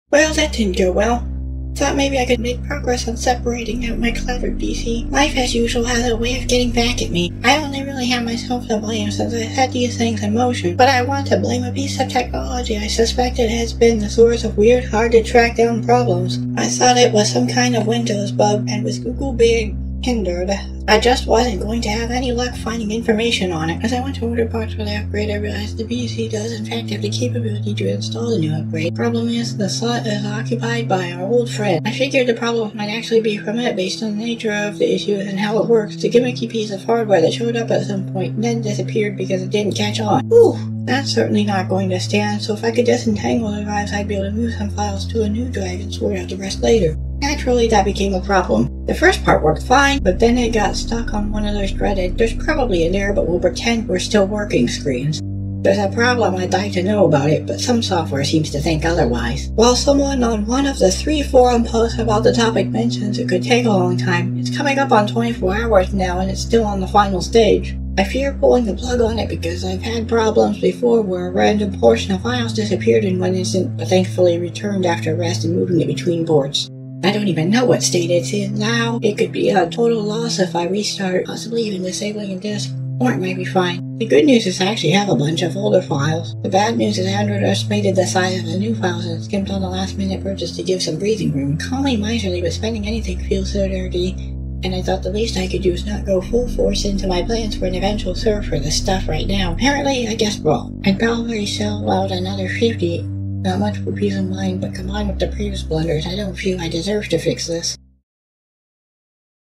Well, that didn't go well. Technology vlog.
Once again turning tragedy into semi-comedic content.
The tale of one stupid polecat messing with unknown powers and immediately regretting it.
I know, this is all my fault, I just wanted things to be better :/

TL;DR undeserved technology upgrade in a race against time results in ongoing disaster.